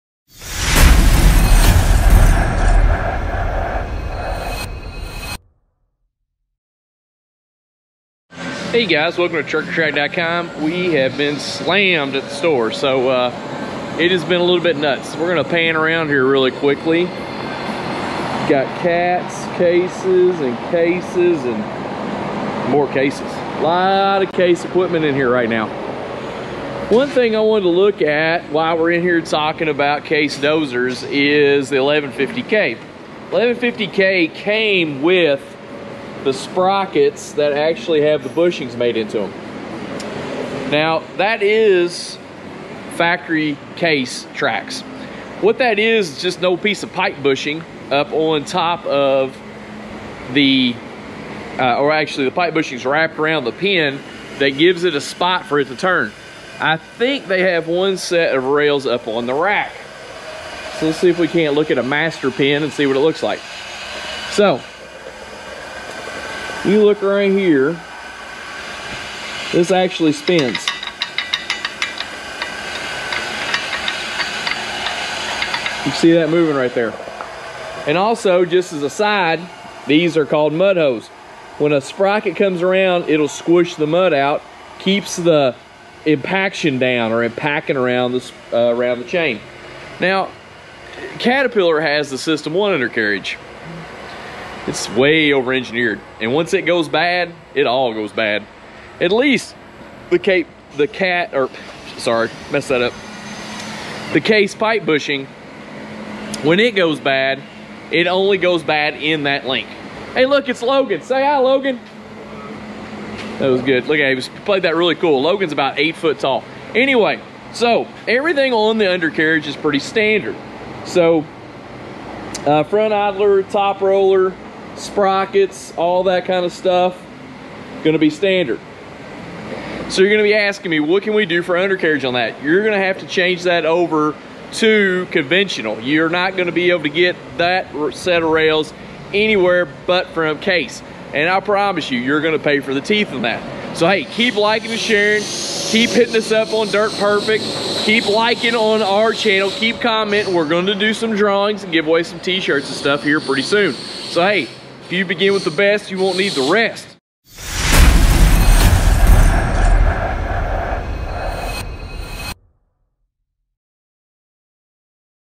Case Extended Life Track Chains Love it or Hate it
Taking a dive into Case Track Chains with Extended life bushings are they worth it? You tell me.